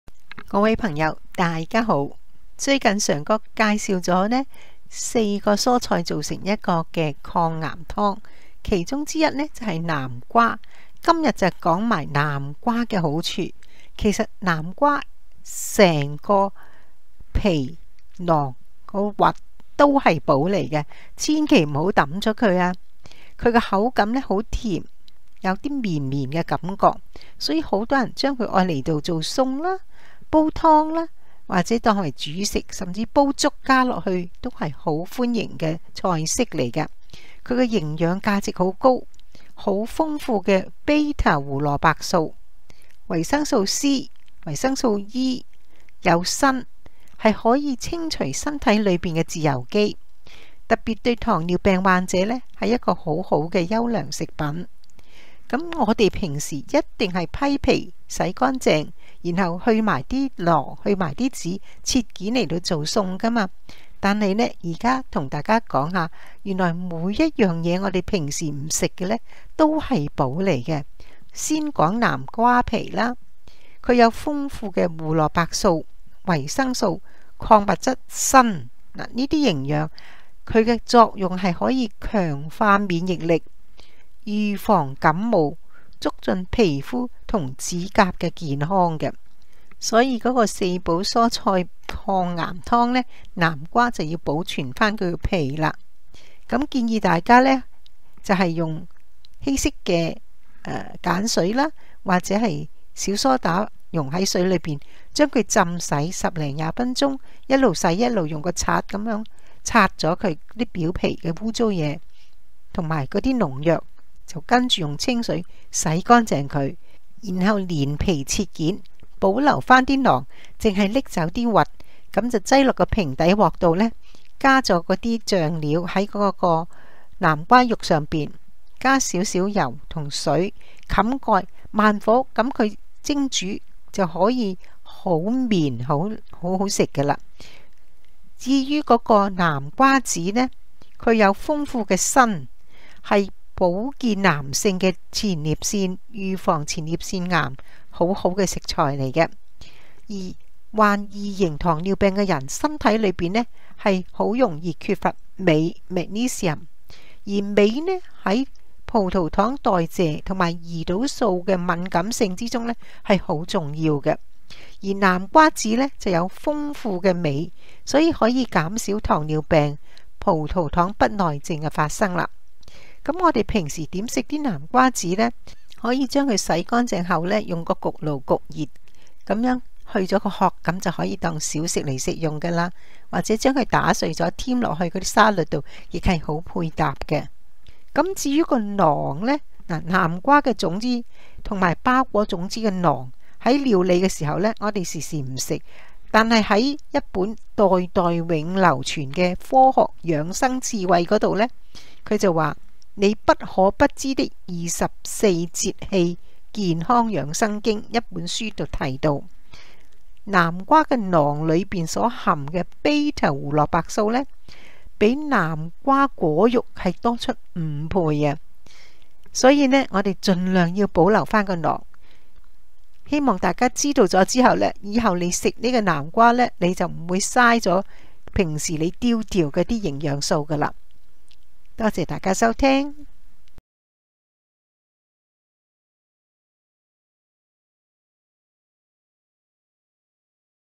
南瓜皮、籽、瓤千萬別丟！
南瓜皮、南瓜籽、南瓜瓤各有好處，棄之可惜！廣告收益除稅外全作行善公益之用！以上健康訊息僅供參考！